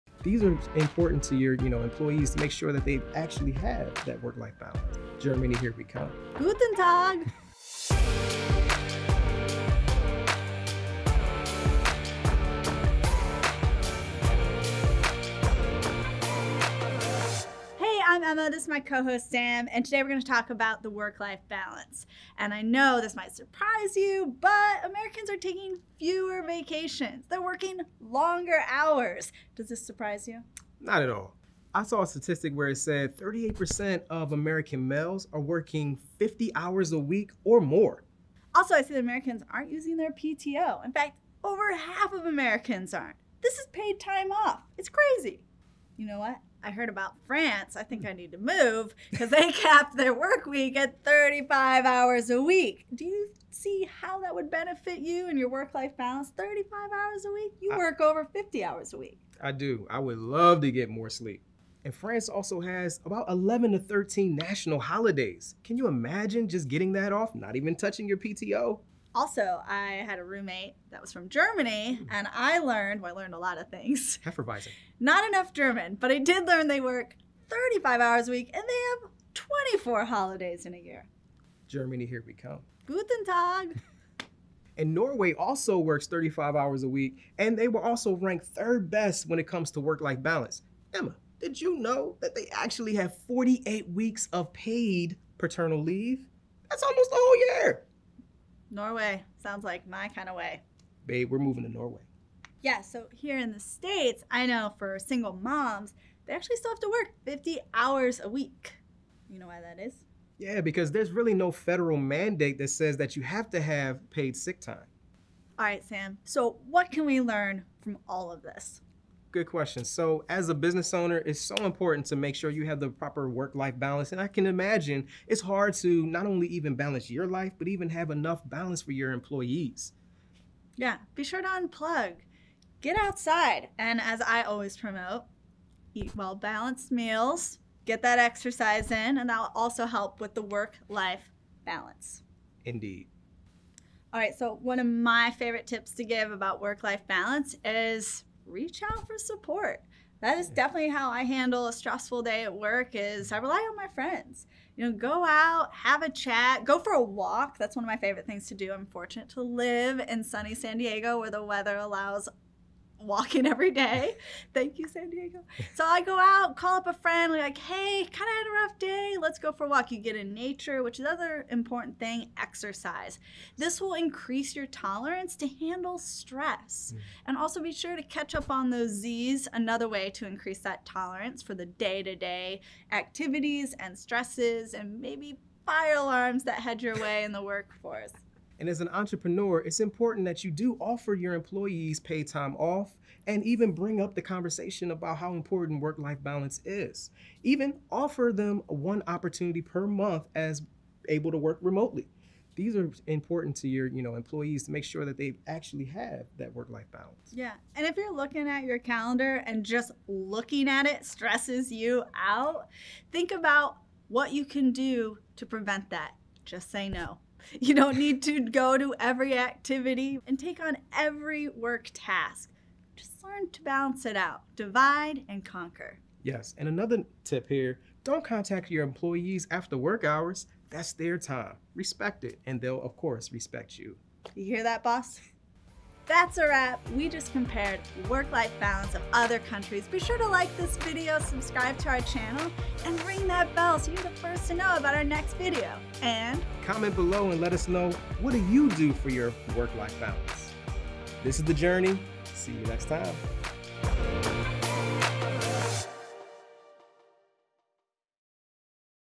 What Entrepreneurs Can Learn About Work Life Balance | The Journey
What Entrepreneurs Can Learn About Work-Life Balance from Other Countries.

⏰TIMESTAMPS⏰
00:45 - Comparison of work-life balance in other countries
01:55 - Work-life balance tips as a business owner

American work-life balance is missing. They are working more, taking fewer vacations and generally falling behind other industrialized nations when it comes to working less and enjoying life more.

American work-life balance remains top of the heap when it comes to the sheer volume of hours Americans put in each week. The majority of workers here put in more than 40 hours per week and there are no laws telling us when to stop either, unlike 134 other countries that set the maximum amount of time workers are clocking in. About one-half of the American workforce won’t use all of their vacations this year, even if it is paid.

The United States is the only industrialized nation in the world that does not legally mandate that employers provide paid vacation time. The entire concept of work-life balance dictates that there is an even balance between the time we spend working and the time we spend doing something else.

But how do other countries balance working and not working? The stats tell us that western Europeans work fewer hours each week than Americans. So, which western European countries are leading the way in work-life balance?

France
France’s labor laws cap the work week at 35 hours and anything more than that is overtime. On average, 15 hours each day are devoted to “personal care and leisure.”
The country also has 11 to 13 national holidays (depending on the region) where offices close. Overtime is less common and it is also not common to take work home on the weekends. Finally, France offers health and social security insurance that covers all illness, retirement, and unemployment.

Germany
Adults in Germany work an average of 26-35 hours a week. Some firms are even moving toward shorter workweeks as well. Germany also has 24 national holidays and collective wage agreements have secured six weeks or more vacation days for employees. Most industries also have collective agreements that limit working hours.

Sundays and national holidays are all mandated as no-work. Rest periods at lunch are the generally accepted norm. Germany also has universal healthcare coverage, paid for through voluntary and compulsory requirements.

Norway
On average, Norwegians work 35 hours per week. Norwegian maternity and paternity leave is also very generous. Parents are entitled to 48 weeks of leave at full pay or 56 weeks at 80 percent pay. Plus, employees in Norway are guaranteed 25 working days of paid vacation each year.

As a small business owner, it is probably difficult to find your own work-life balance, let alone making sure your employees have achieved it.
Why does American work-life balance matter? Working excessively is bad for your heart, according to the latest research. By encouraging employees to take more time off, the small business owner will reap rewards like less employee sick time and higher employee engagement.
For small business owners seeking their own work-life balance, this reward will also lessen the stress all entrepreneurs feel. While this won’t solve all of your problems, it should give you a nice head start.